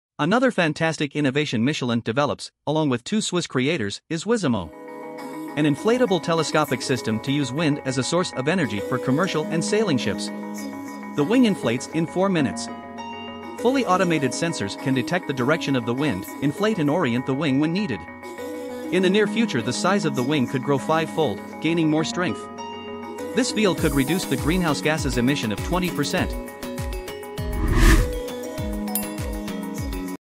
Michelin Innovation - Inflatable Wing Sail to reduce greenhouse gases emissions - WISAMO
Wisamo (WIngSAilMObility), Inflatable Wing Sail, an innovation by Michelin for commercial and sailing ships said to reduce greenhouse gases emissions of 20%.